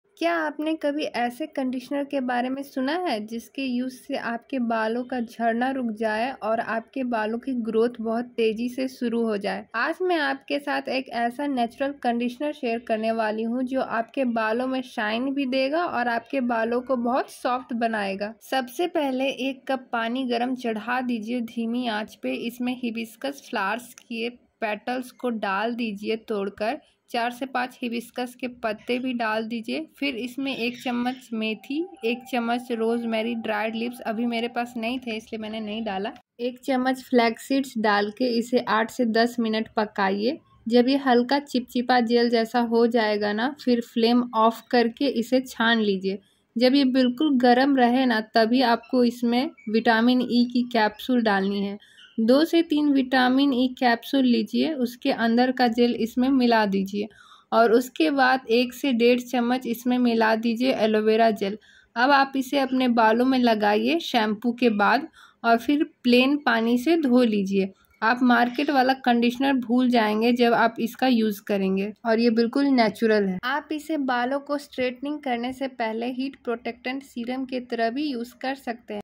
Homemade Conditioner & Heat protector serum | chemical free | 100% Natural
Hi everyone
In today's video I am going to tell you about how to make DIY homemade conditioner and heat protector serum. You will need few kitchen ingredients to make this and this is totally chemical free and 100% natural. This conditioner will make your hair soft,smooth,Shiny and healthy that you will forget about market conditioners.

You can make this conditioner easily at home by following a few steps which I have mentioned in the video so watch the video till the end to know how to make this homemade conditioner and heat protector serum.
If you have any query you can ask me in the comment section.